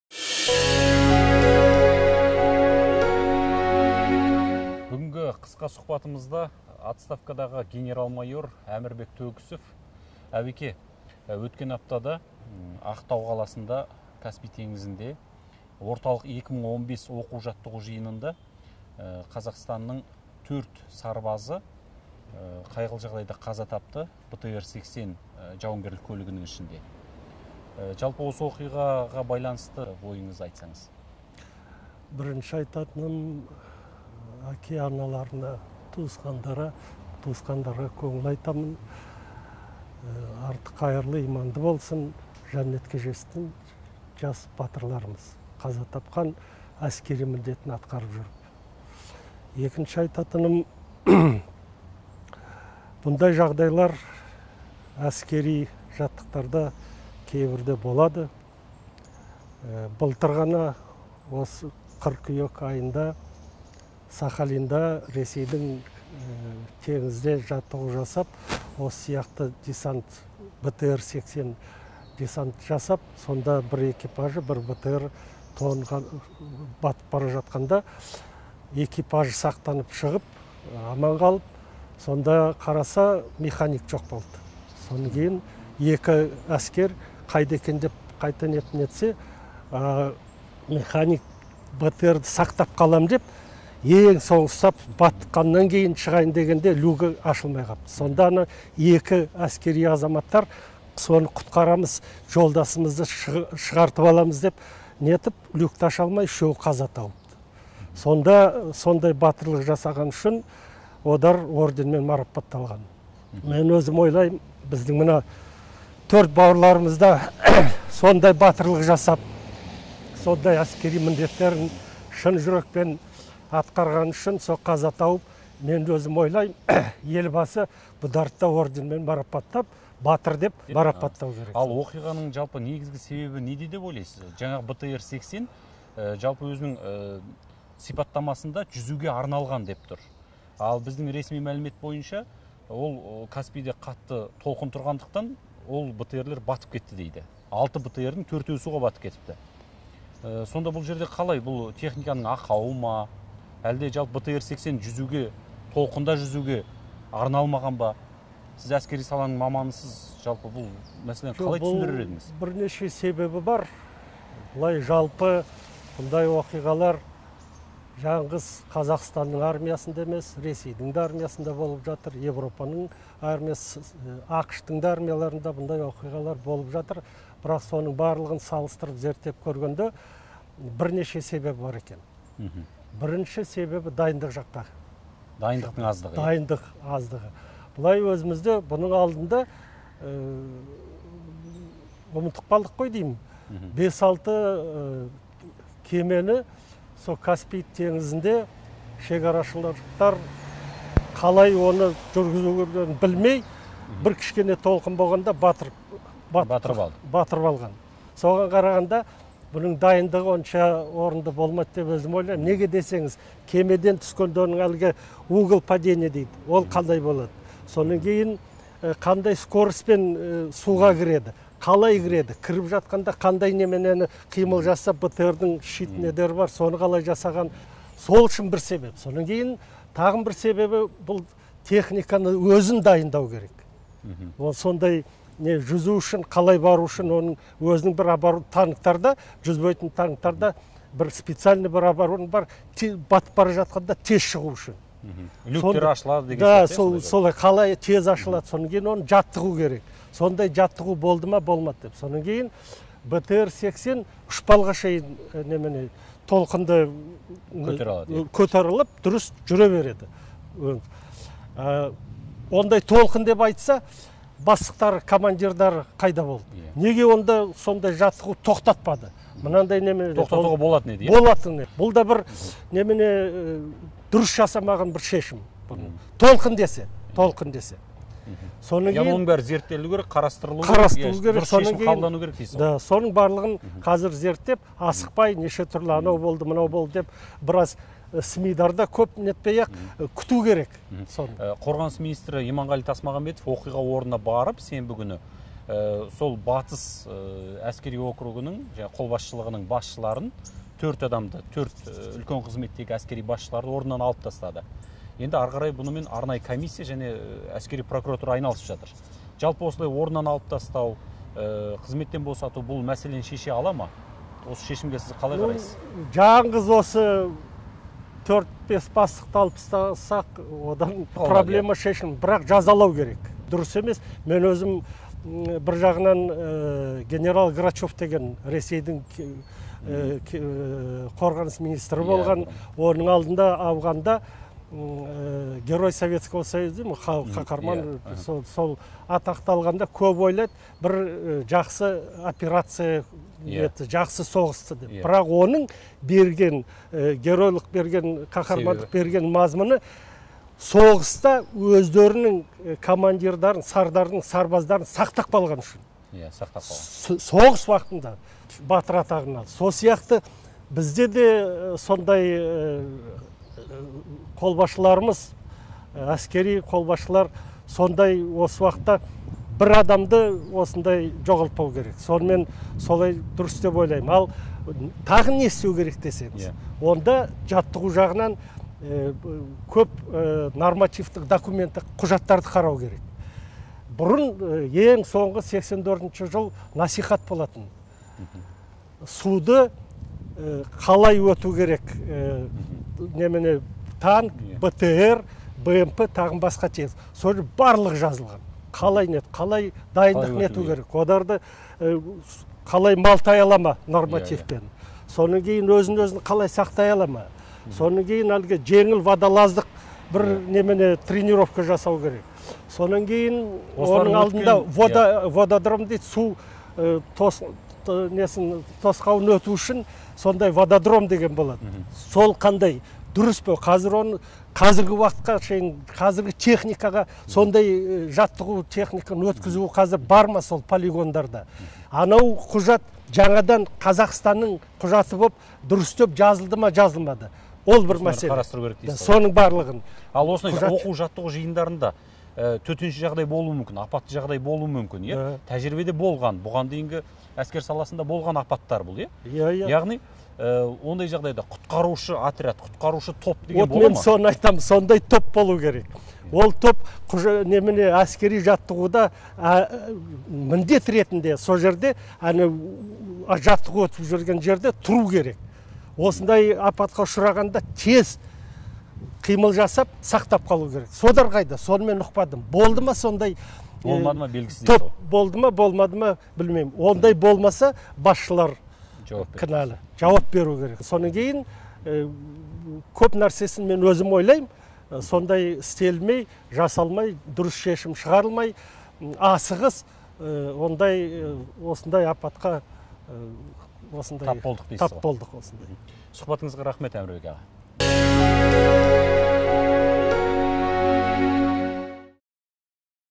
Сарбаздар неге мерт болды?
Қазақстан қорғаныс министрінің бұрынғы орынбасары Каспий теңізінде өткен әскери жаттығуда төрт сарбаздың қаза табуына қатысты пікірін айтты.